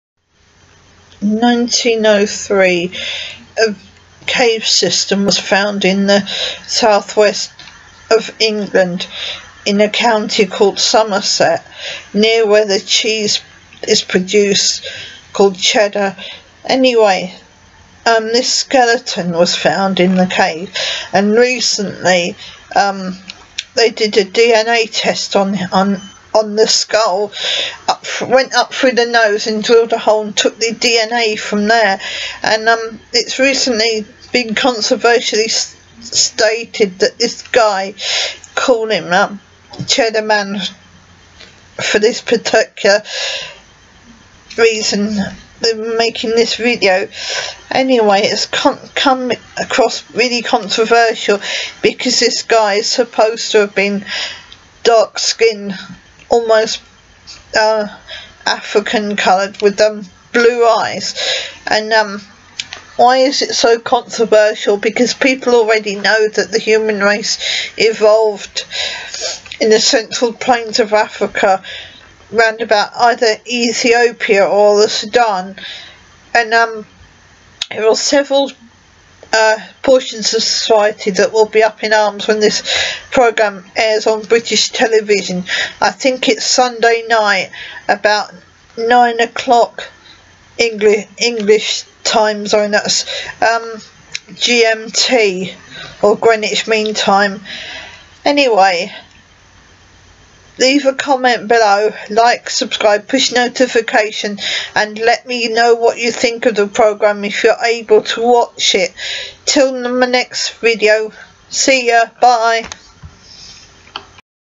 Is Cheddar Man really black?
Personally i think that he is because of where humans evolved. What I find strange is that he had blue eyes.